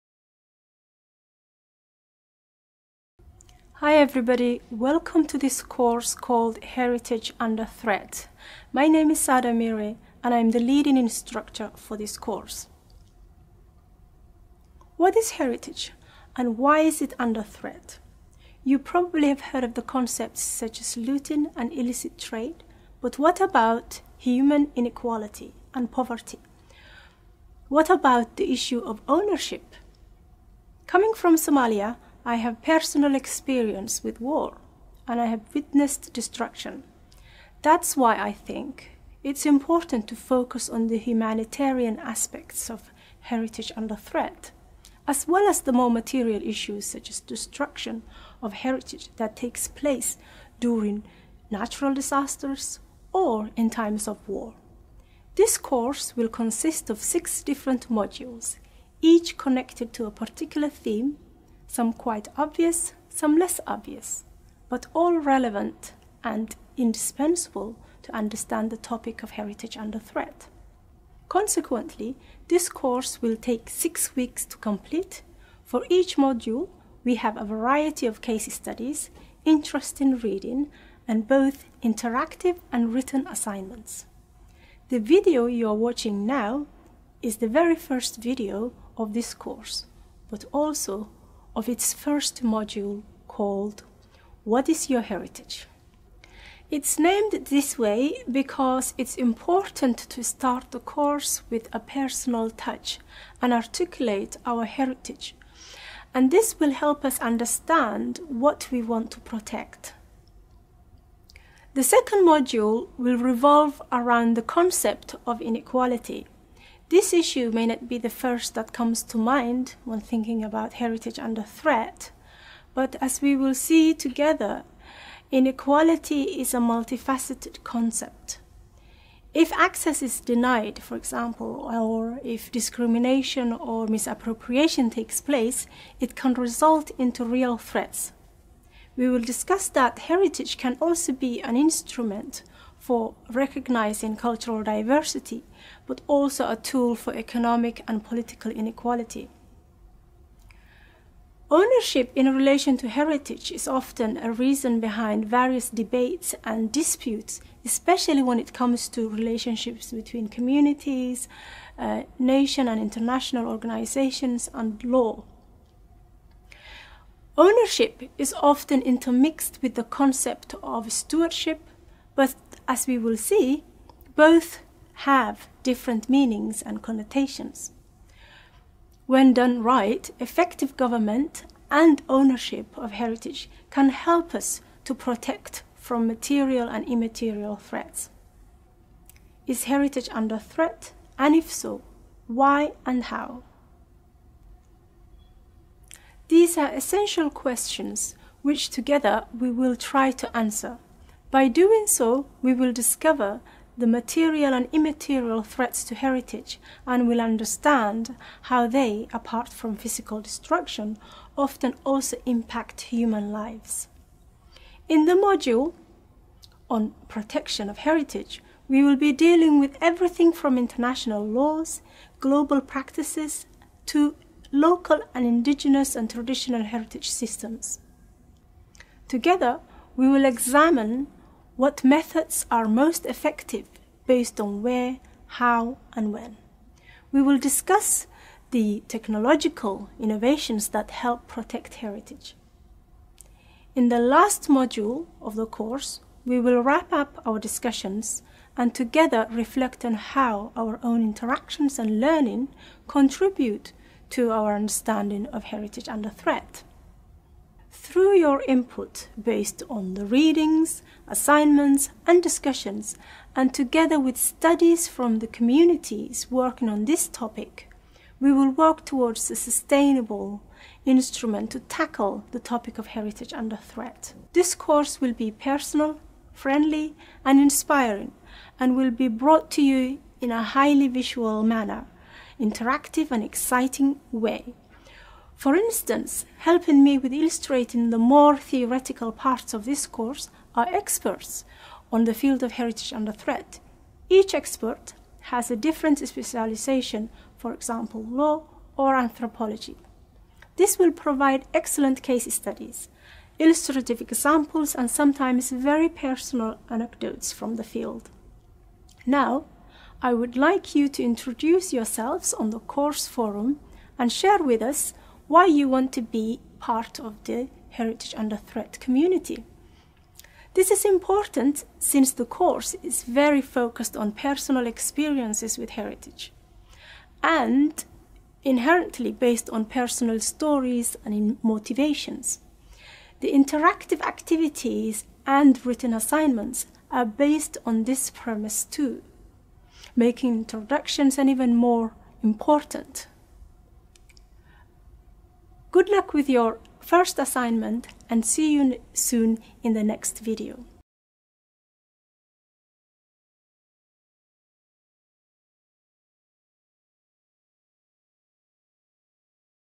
1.1 Heritage Under Threat- course introduction with dr. Sada Mire
What is your heritage? Who defines heritage? Why is heritage under threat? How can we protect heritage?
This new Leiden University course is presented by Dr. Sada Mire from the Faculty of Archeology and the LDE Centre for Global Heritage and Development.